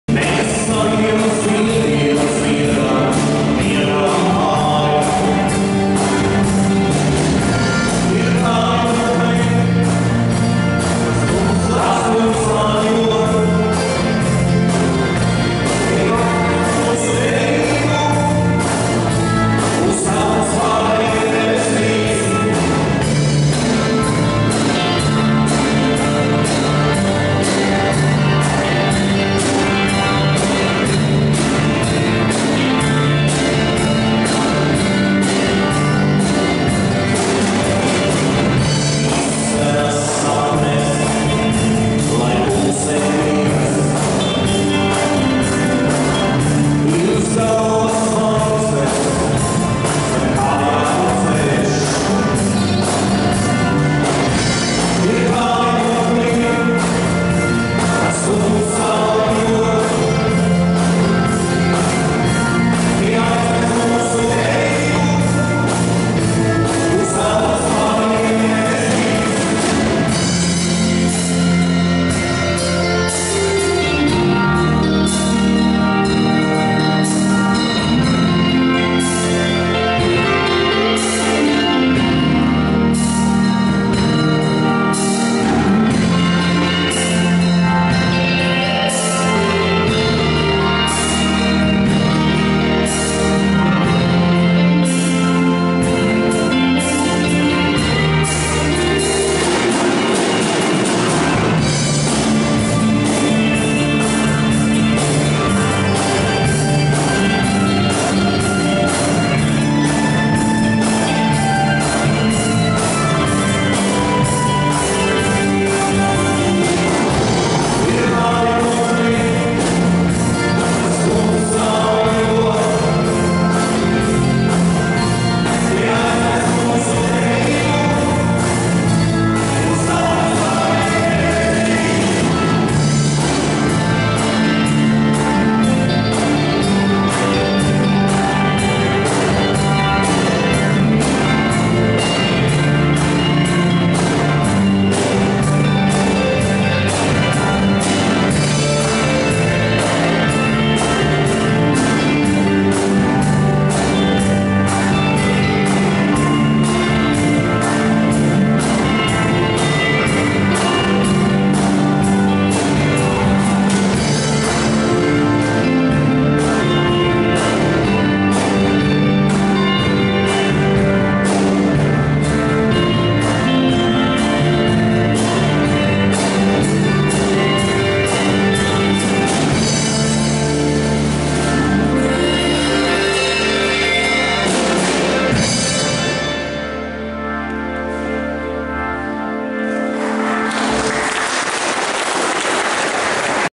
Grupa "Soli" Live in Concert. Riga, 1.8.2017 # Part 1
Die Gruppe Soli trat im Rahmen der Ehrung und der Andacht mit ihrem Konzert für den jung verstorbenen Künstler Uldim Pars Balodim auf.